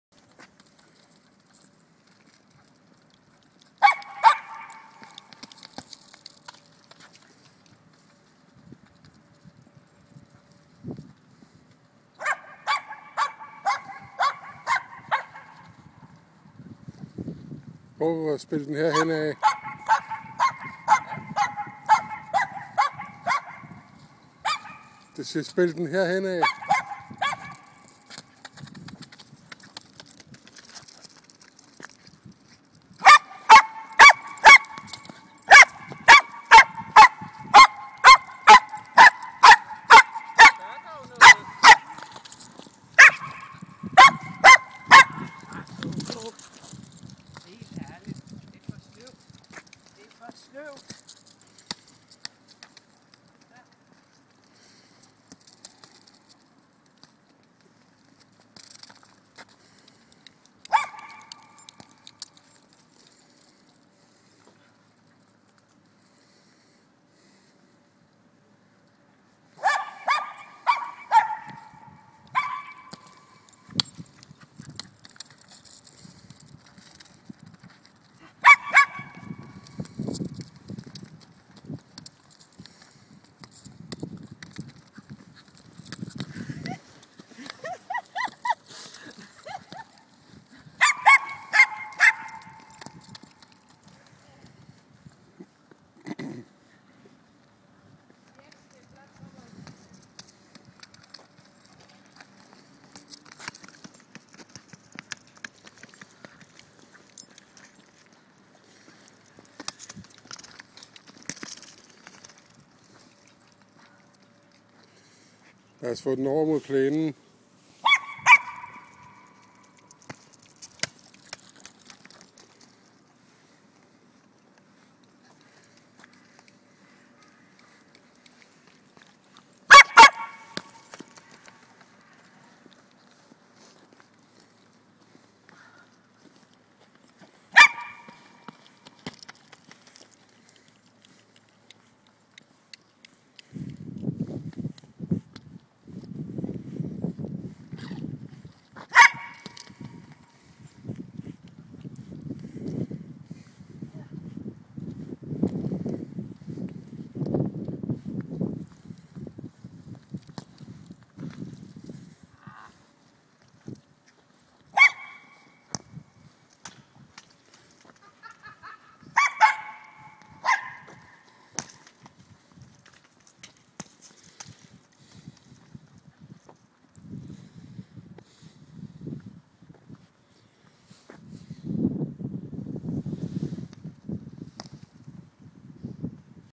Pogo Spiller fodbold
Loco Pogo udviser formidable evner ud i fodboldspil. Og det der med at drible er altså ikke noget vi har lært hende.